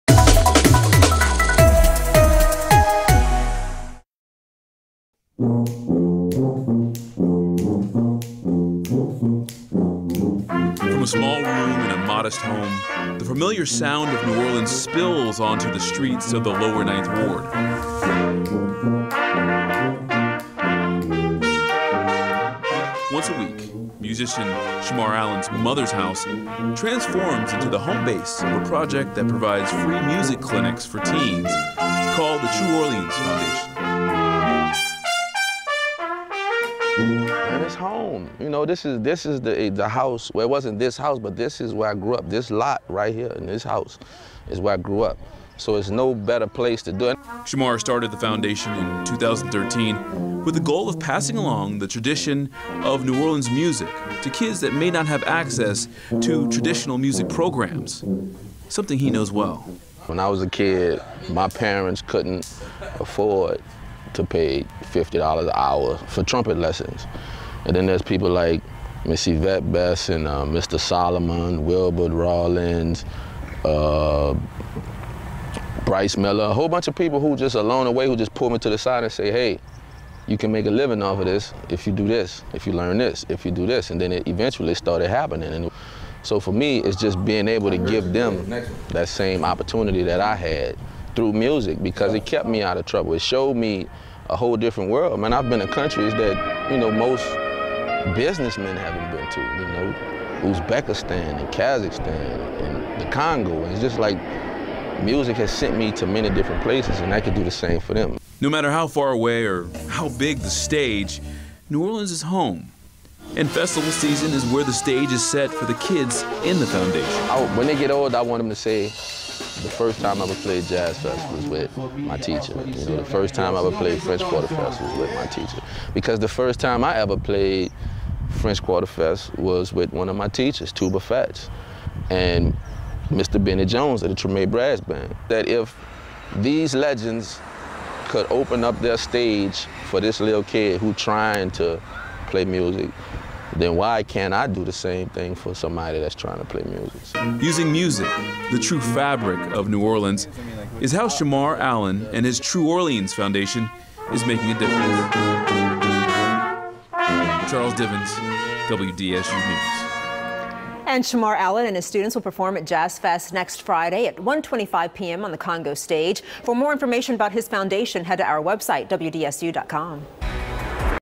Making A Difference: Project aims to help next generation of NOLA musicians keep music alive
With Jazz Fest in full swing, the impact music has on the Crescent City is front and center. WDSU anchor Charles Divins introduces you to a project that aims to help the next generation New Orleans musicians keep the music alive.